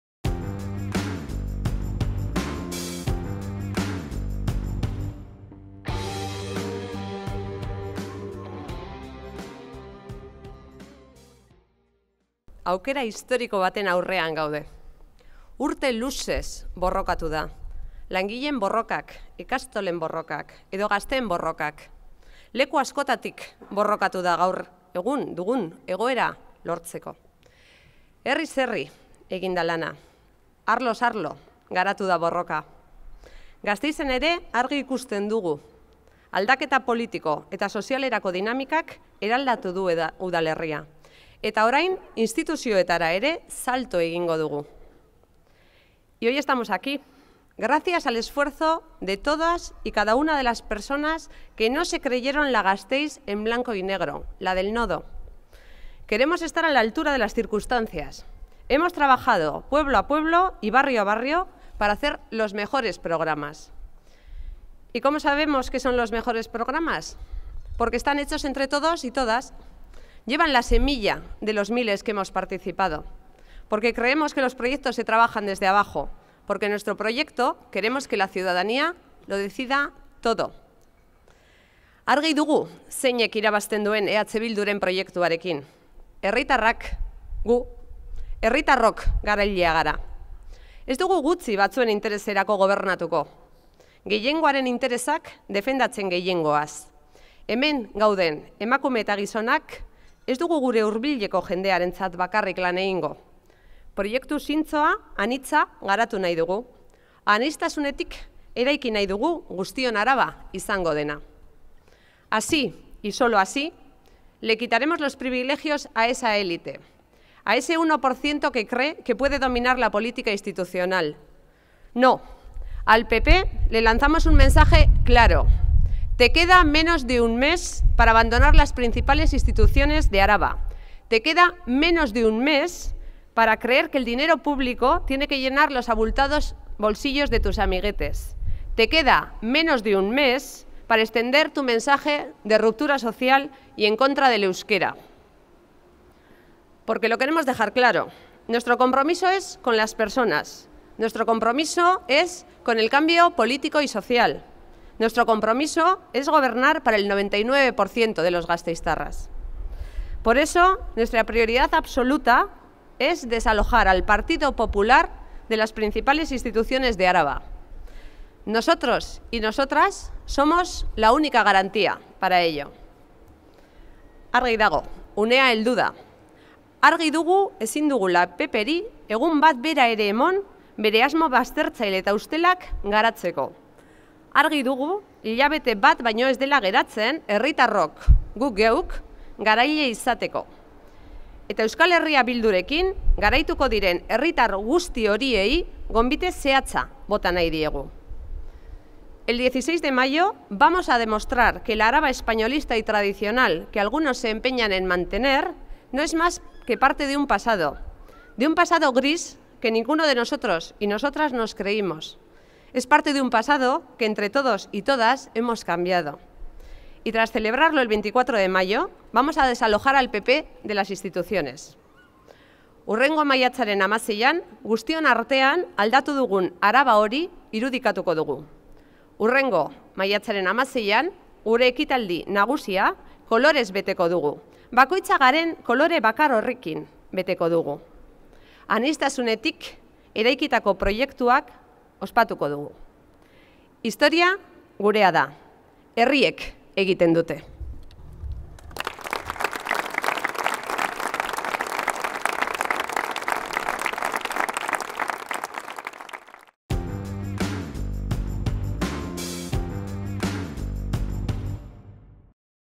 Larrion: "Gure lehentasun nagusia PP Arabako instituzio nagusietatik kentzea da"
Miren Larrion participa junto a las cabezas de lista de los municipios de Araba en la presentación de las candidaturas municipales.